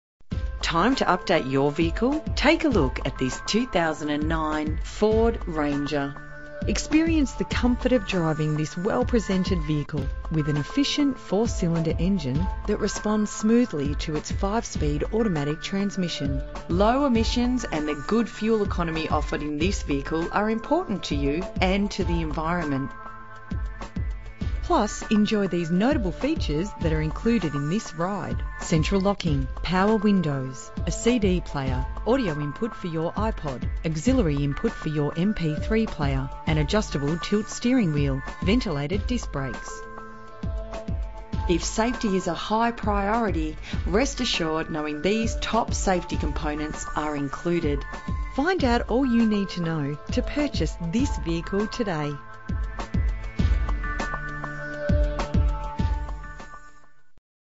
2009 FORD Ranger XL High Rider - Deer Park VIC
2009 FORD Ranger XL High Rider 3 litre Automatic Utility Silver Exterior.